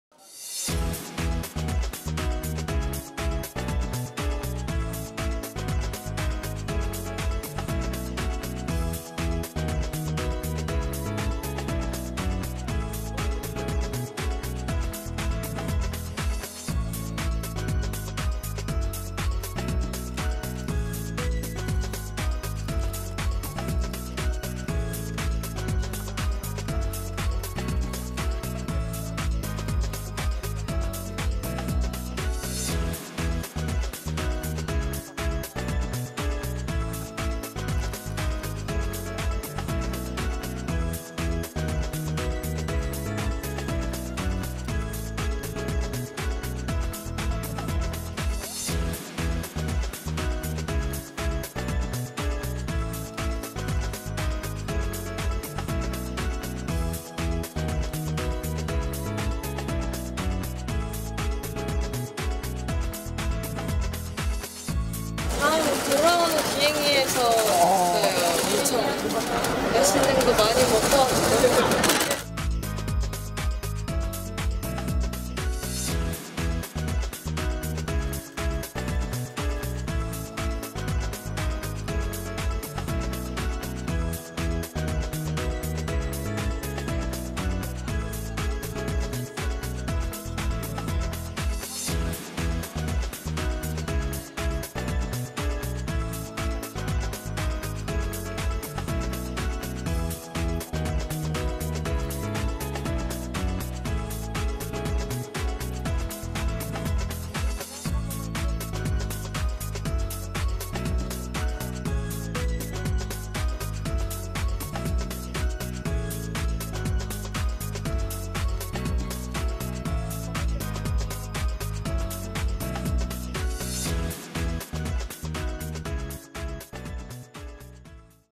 아이유(IU), 밀라노 사로잡은 '청순 요정'··· '사랑스러움 가득' [비하인드]
[비하인드=김태석기자] 가수 아이유(IU)가 22일 오후 해외 패션쇼 일정을 마치고 인천국제공항을 통해 입국했다.